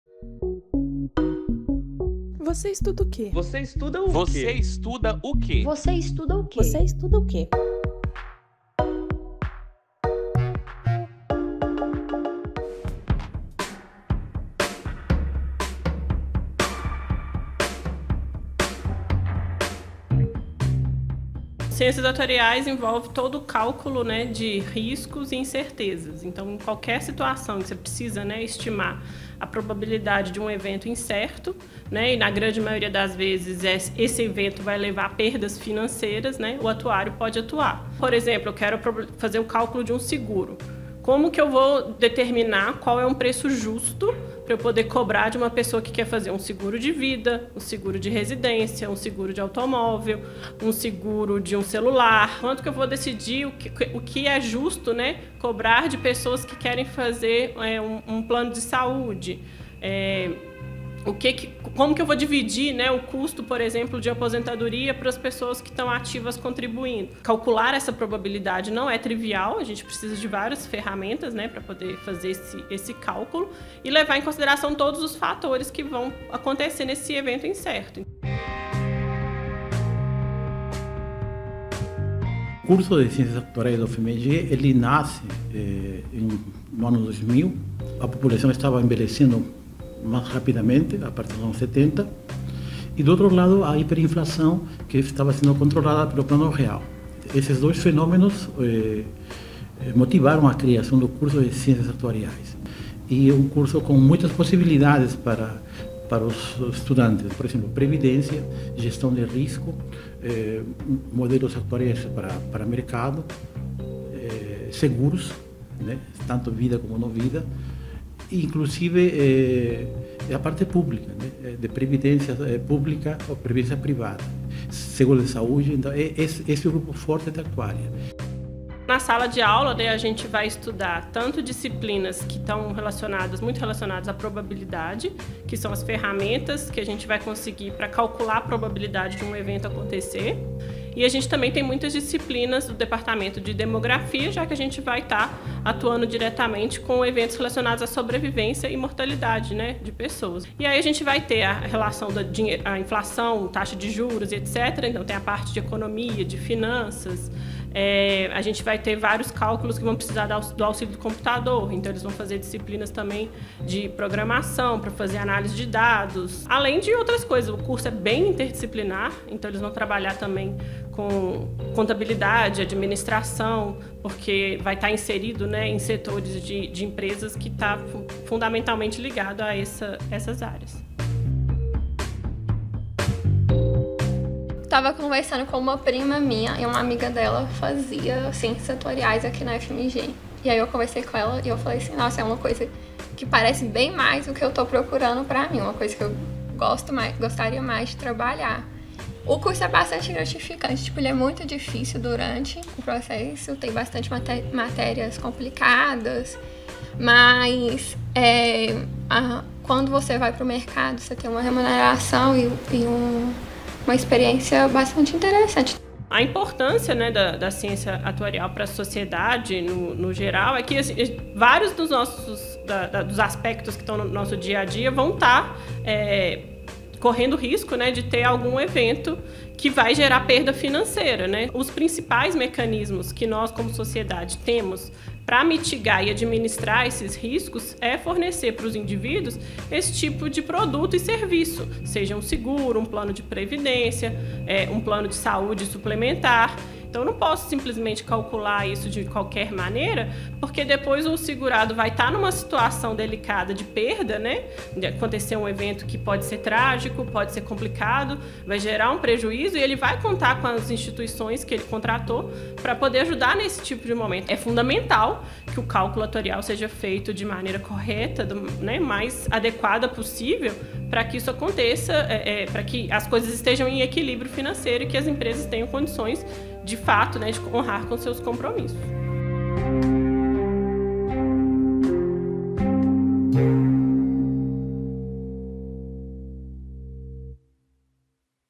Conheça o curso de graduação em Ciências Atuariais da UFMG
Criado em 2001, o curso de graduação em Ciências Atuariais da UFMG busca formar profissionais em gestão de riscos e análise financeira. Seja em curto ou longo prazo, um atuário auxilia pessoas e instituições a se prepararem para o futuro.

Para entender melhor sobre o ramo, a TV UFMG conversa com professores e estudantes do curso! O vídeo faz parte da série "Você estuda o quê?”, que busca divulgar cursos da Universidade diferentes ou pouco conhecidos.

Ficha técnica:
Produção: Amanda Gomes
Reportagem: Amanda Gomes e Vitória Brunini
Imagens: Ângelo Araújo e Lucas Tunes
Videografismo: Bia Campos
Edição de imagens: Otávio Zonatto
Edição de conteúdo: Vitória Brunini

Entrevistados/as:
Thais Paiva – profa. Depto. Ciências Atuariais – ICEx/UFMG
Fredy Castellares - coord. curso Ciências Atuariais – ICEx/UFMG
Ana Clara - estud. Ciências Atuariais – ICEx/UFMG